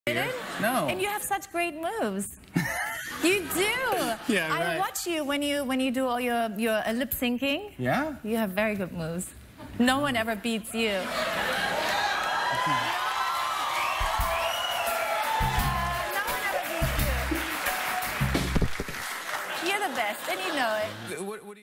Jimmy Fallon Knocks Over His Microphone
Jimmy Fallon Knocks Over His Microphone during the interview with Heidi Klum.